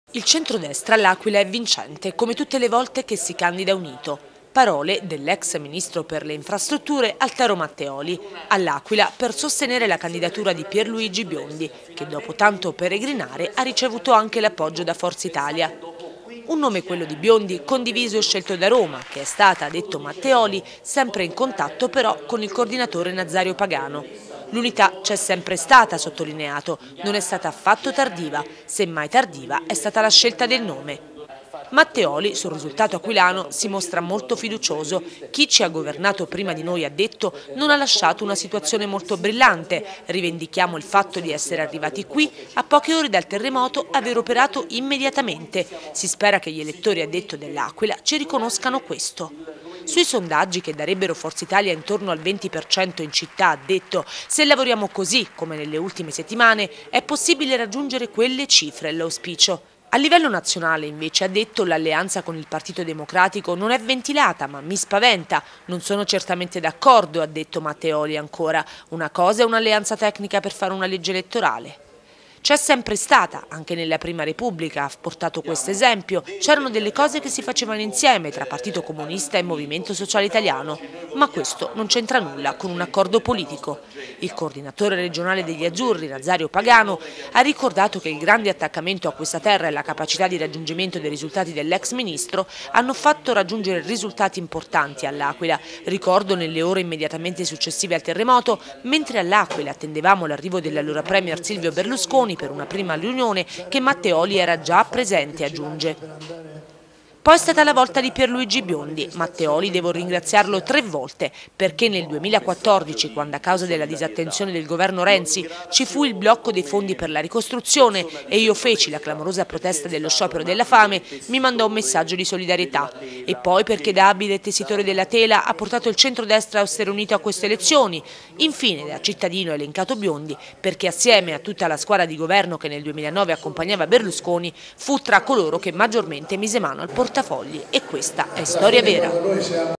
Matteoli a L’Aquila a sostegno di Biondi
L'ex ministro per le infrastrutture Matteoli a L'Aquila per sostenere la candidatura di Biondi. Il centrodestra sarà vincente perchè unito ha detto